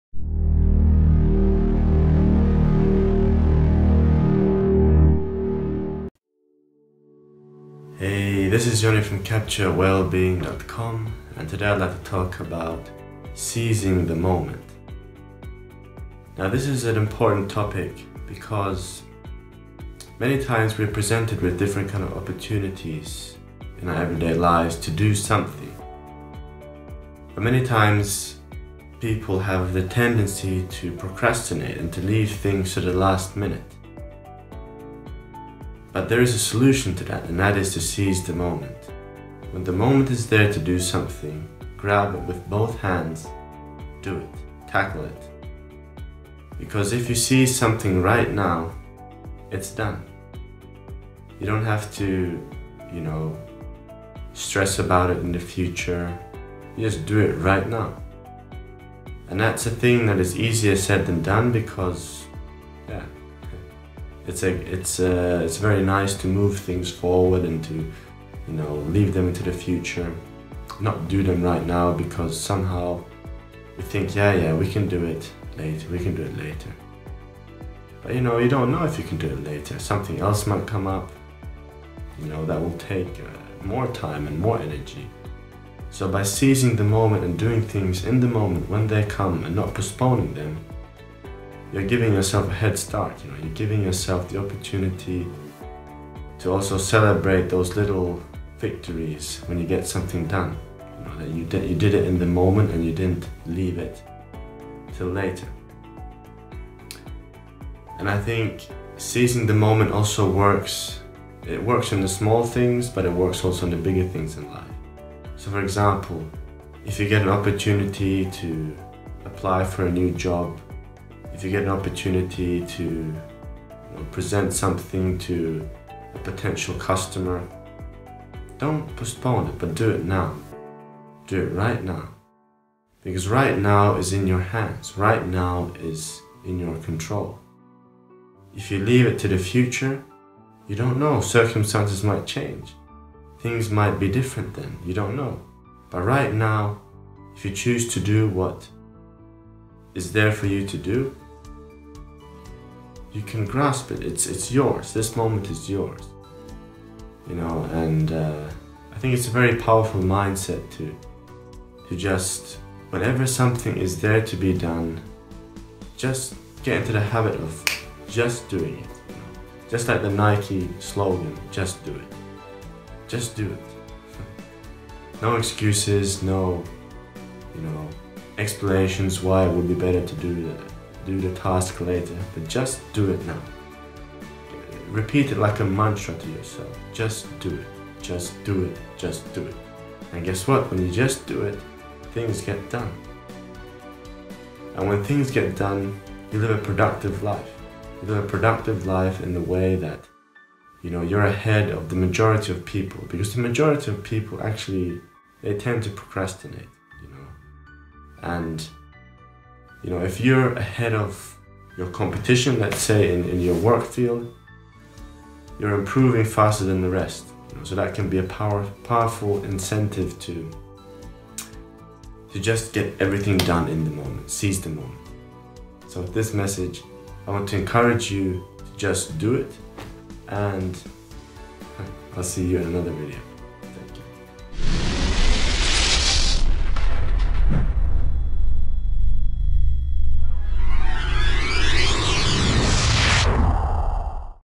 Seizing the moment
Taking action right now is a key to more productive and fulfilling life.